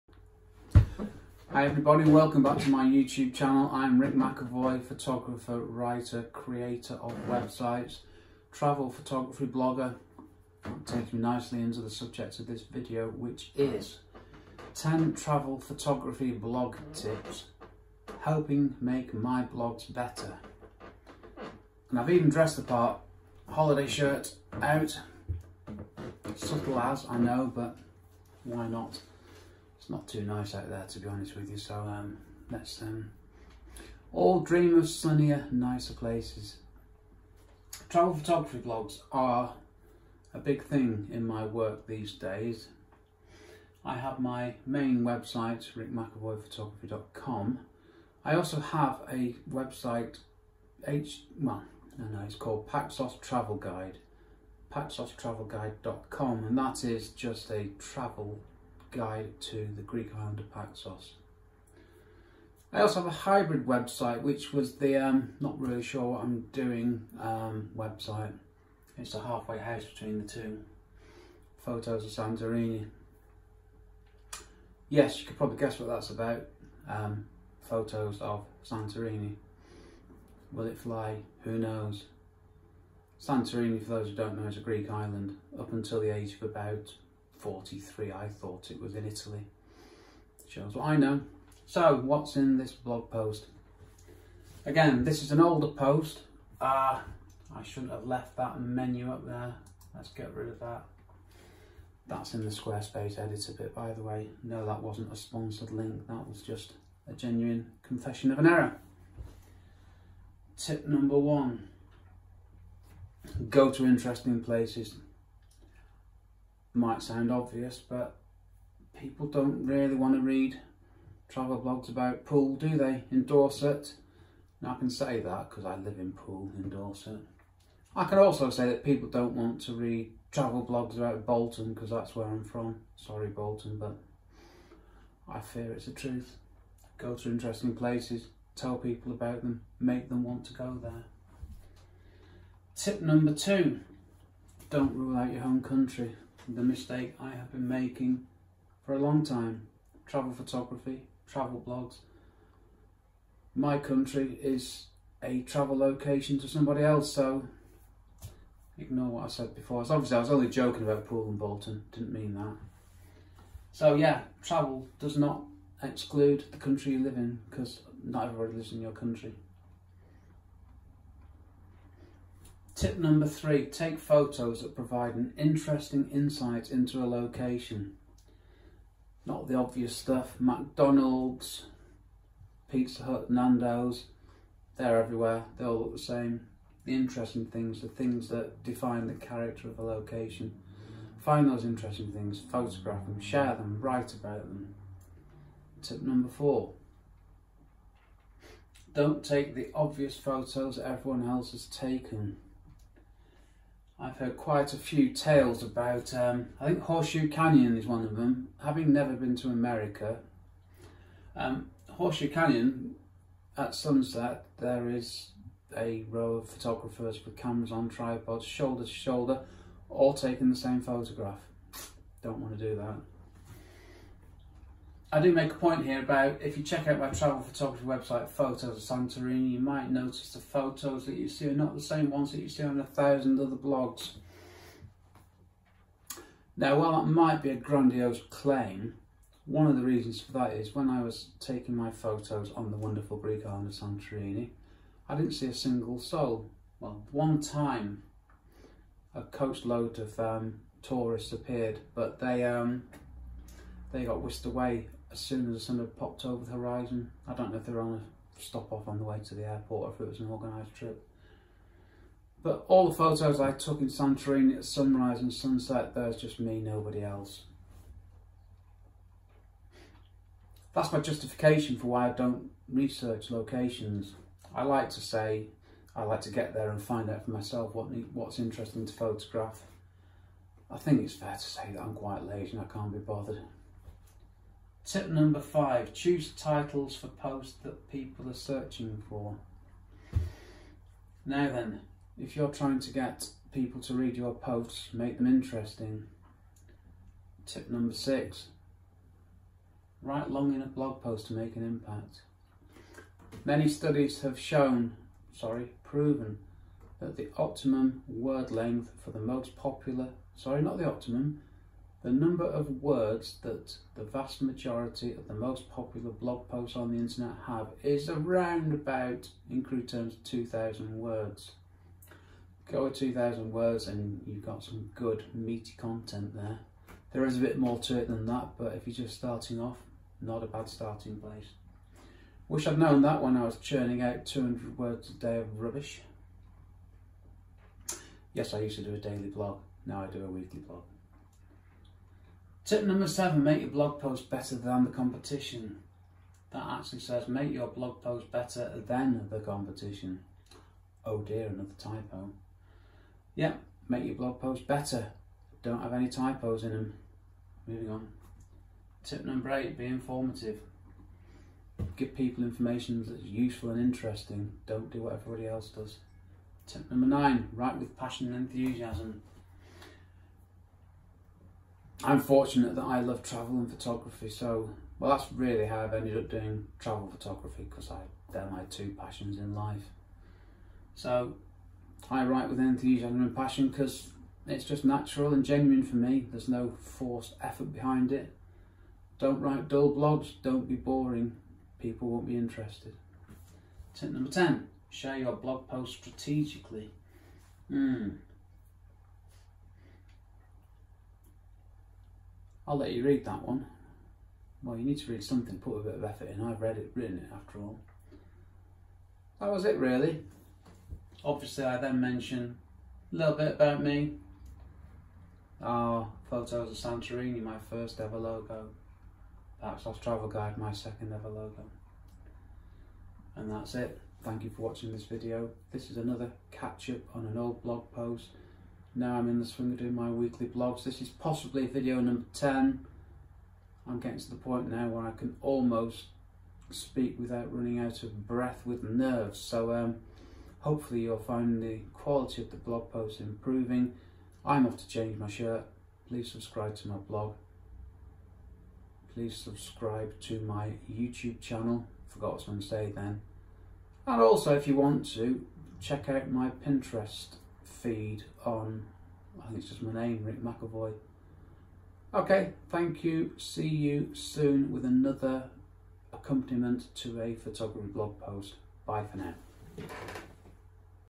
10 Travel Photography Blog Tips Helping Make My Blogs Better - And check out my funky shirt!!
I write travel photography blogs these days. And that gave me a thought for this blog post.

What would my top 10 travel photography blog tips be? How I can help people create better blogs? It was good to think about this and the work that I have done to date. So in this post I am going to share the 10 tips that will help you and I create better travel photography blogs, and hopefully more engaging, interesting and popular travel photography blog content.

Some of these tips are things that I have learned and do well, some are things that I have to work on myself to improve the quality of my writing.

At the end of this post I will tell you about my travel photography blogs.

Ok - let’s get to it!